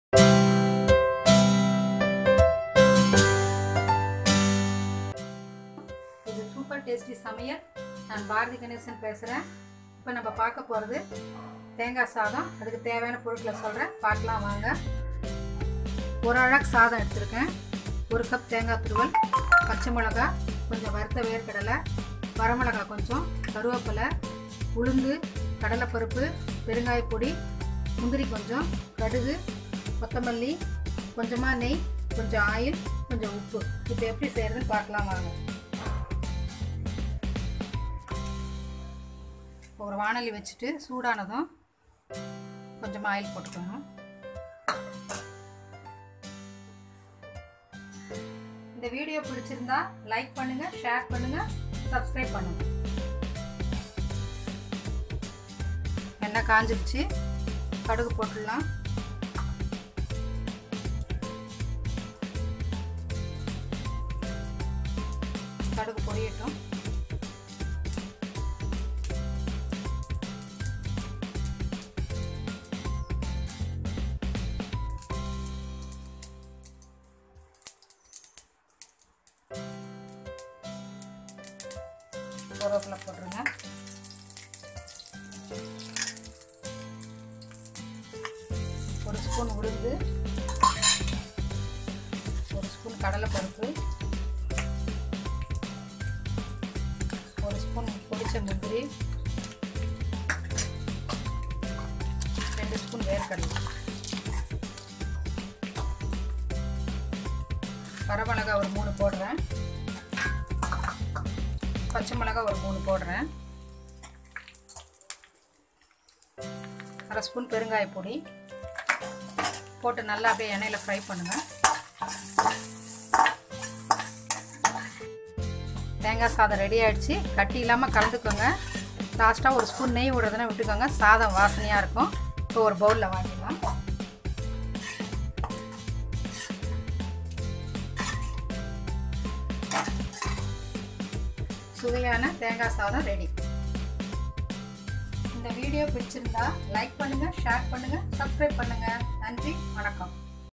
Thengai Sadham Recipe || How to make thengai sadham in tamil
Hi friends in this video i will show you how to make thengai sadham in tamil